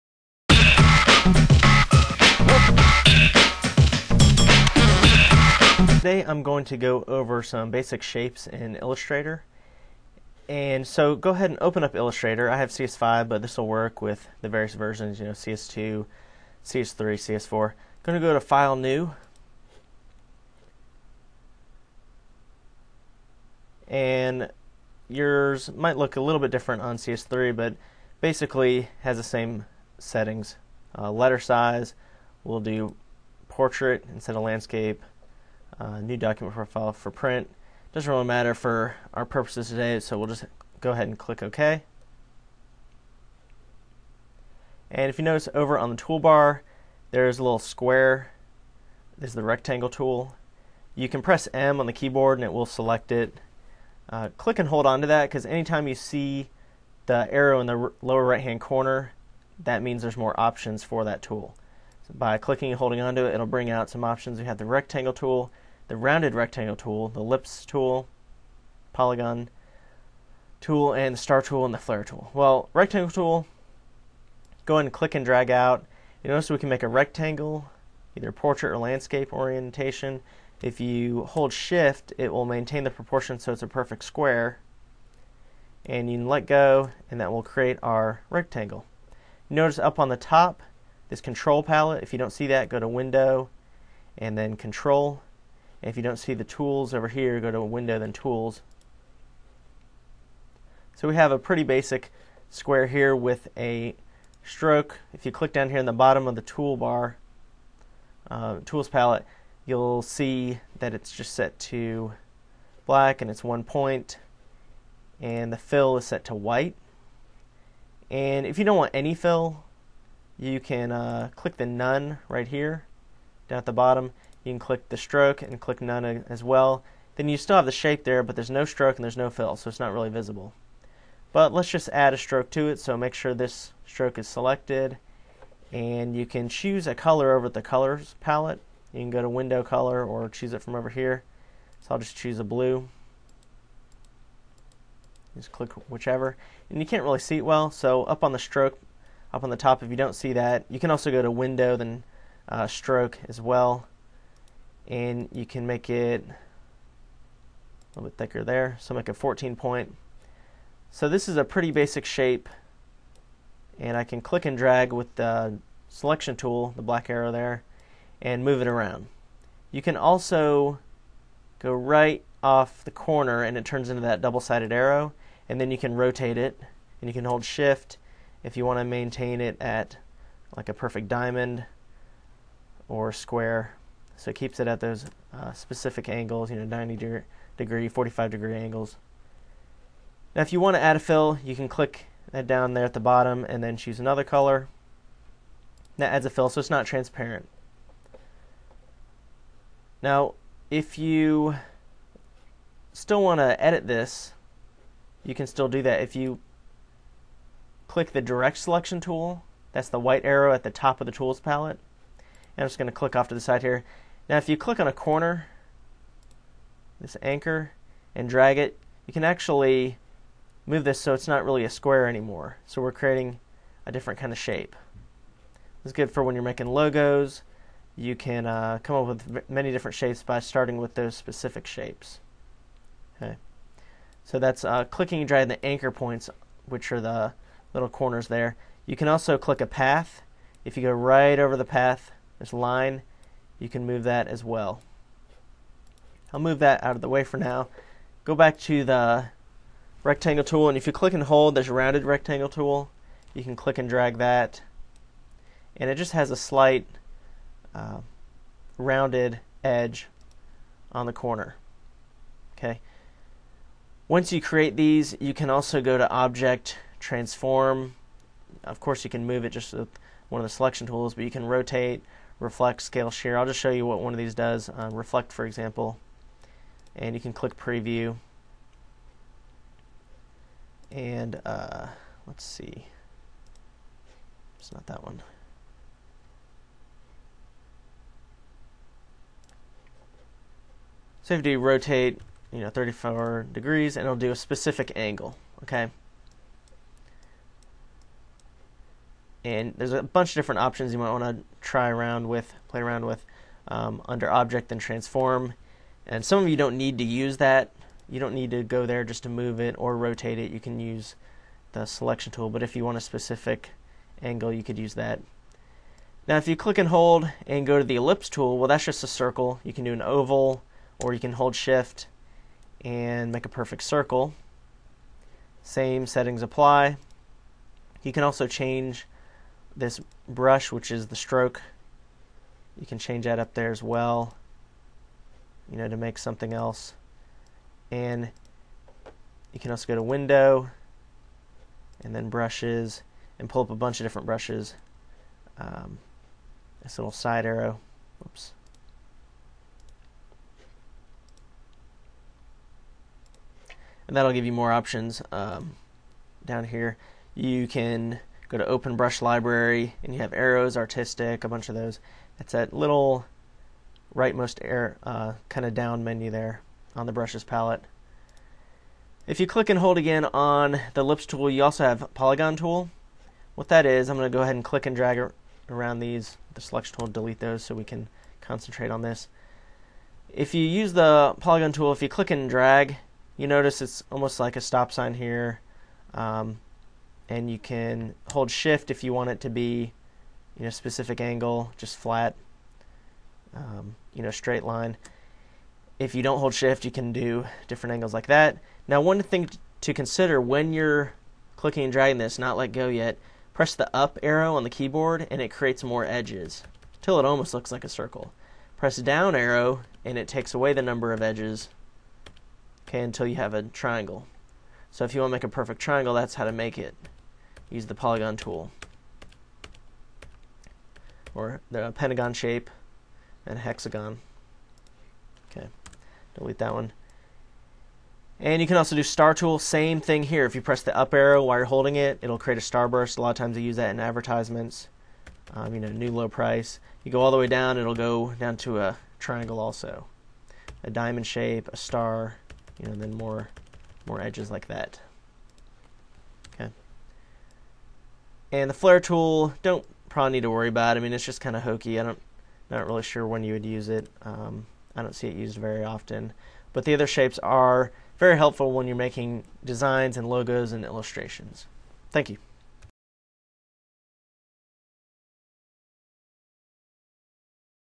Introduction to Adobe Illustrator Shape Tools
Intro to Adobe Illustrator Shape Tools, such as the rectangle tool, rounded rectangle tool, elipse tool, polygon tool, and star tool.